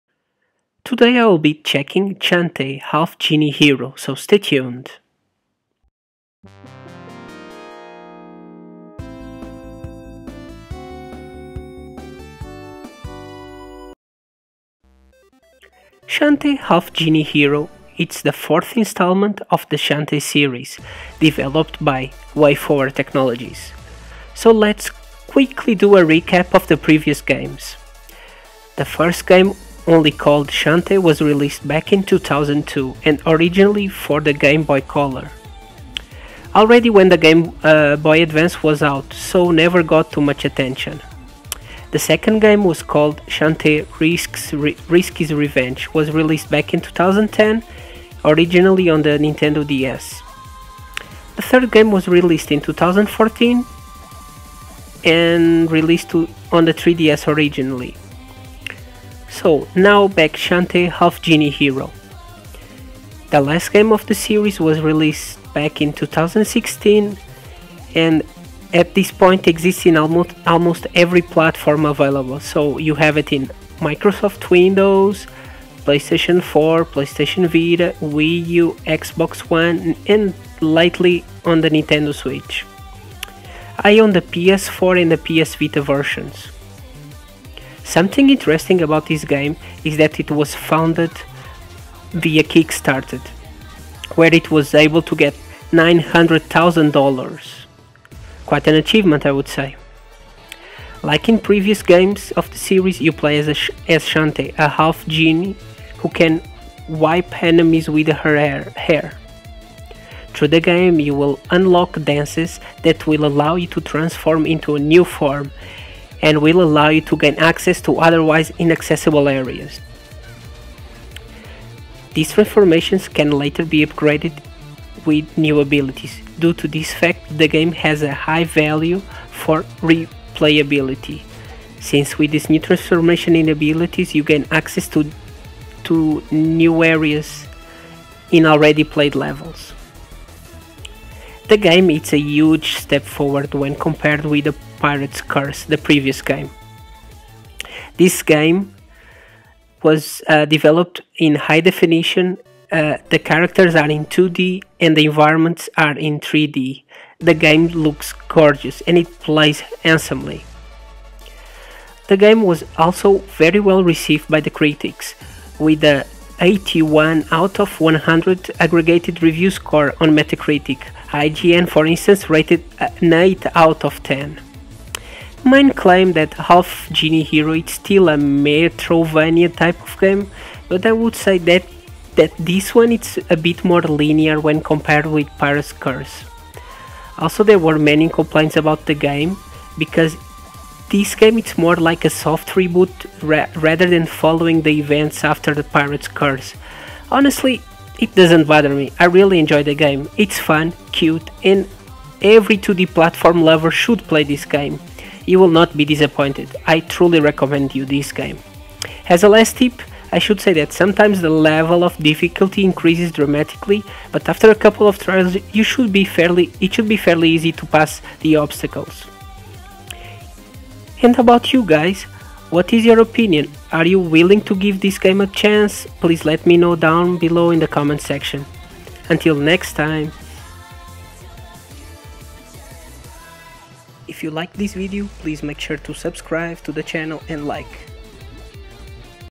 Shantae Half Genie Hero ... Good?
Today I will be talking about Shantae: Half-Genie Hero its the forth instalment of the Shantae series, developed by WayForward Technologies.

Are you willing to give this game a chance? Please let me know down bellow in the comment section.